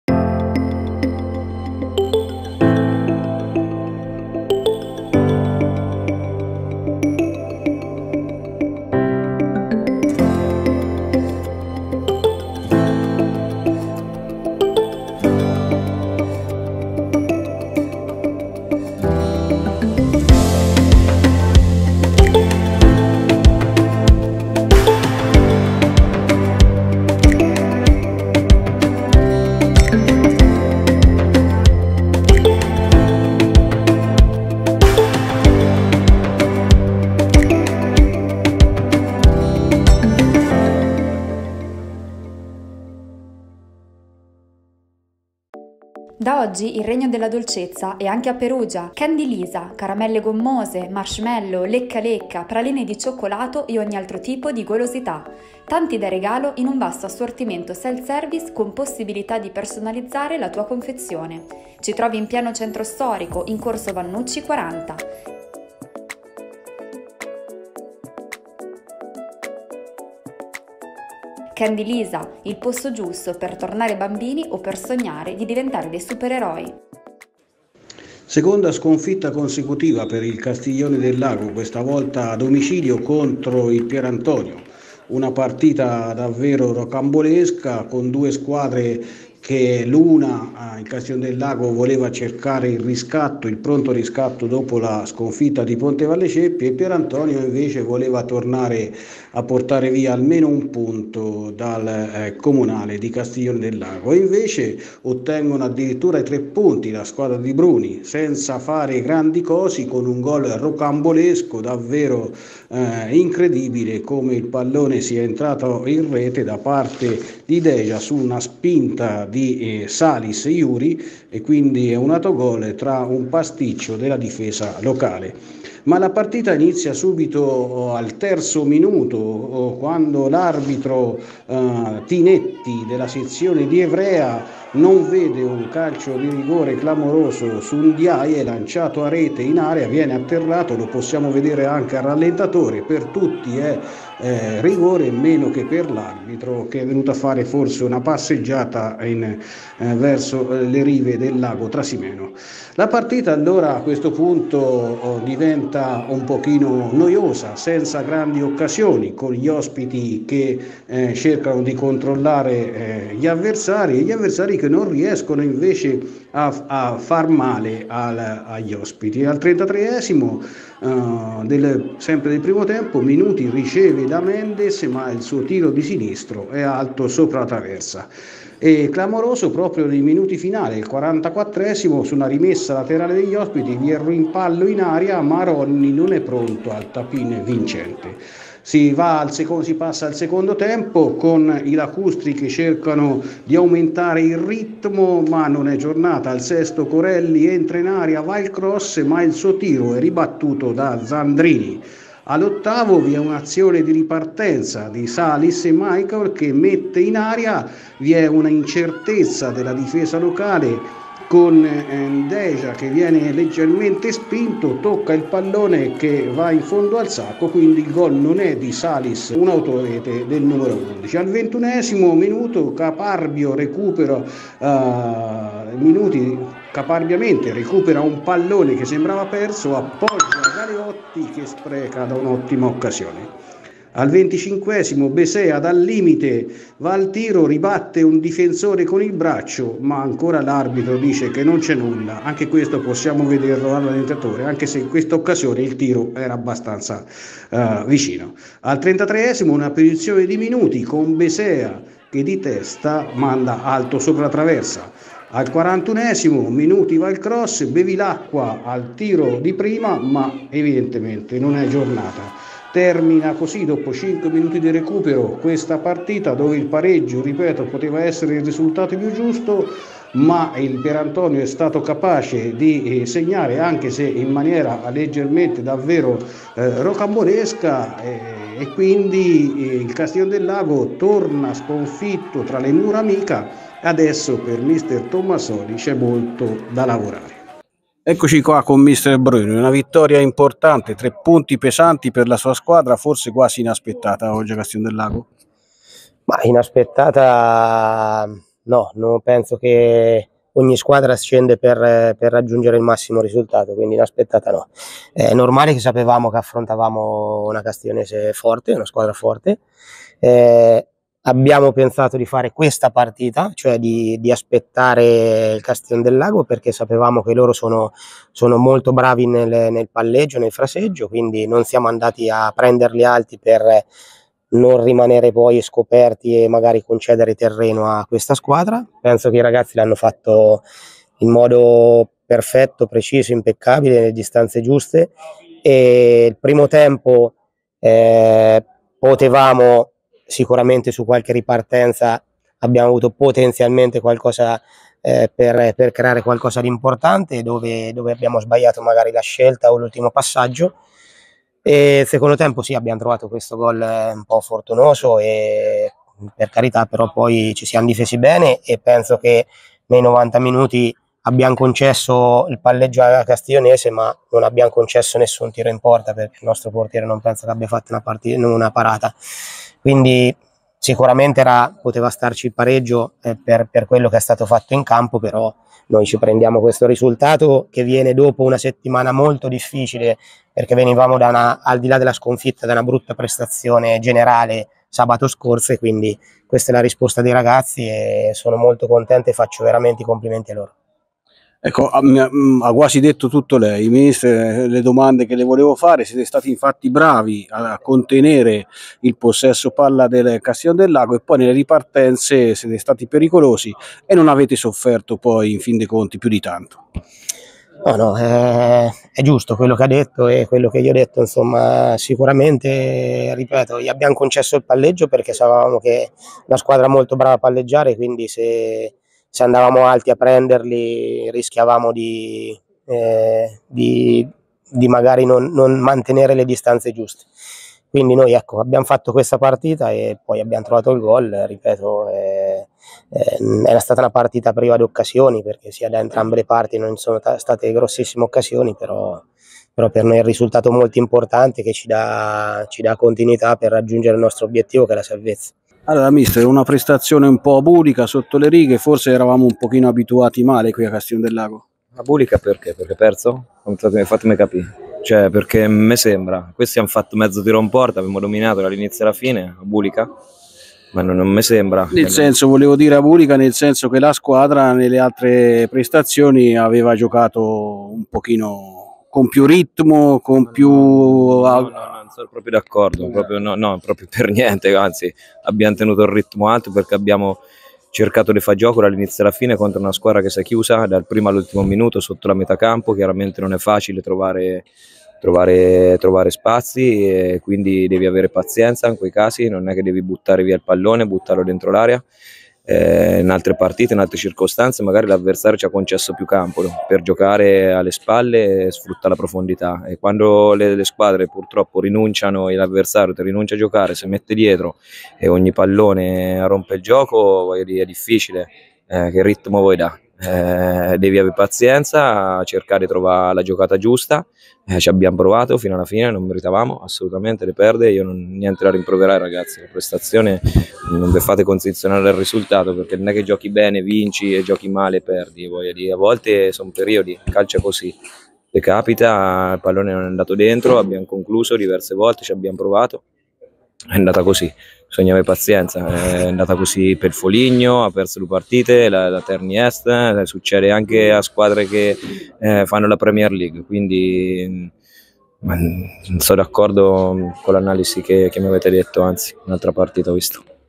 CASTIGLIONE DEL LAGO-PIERANTONIO 0-1 ECCELLENZA 28 gennaio 2024
Campionato di Eccellenza - 19° giornata
Servizio di Giorgio Fanfano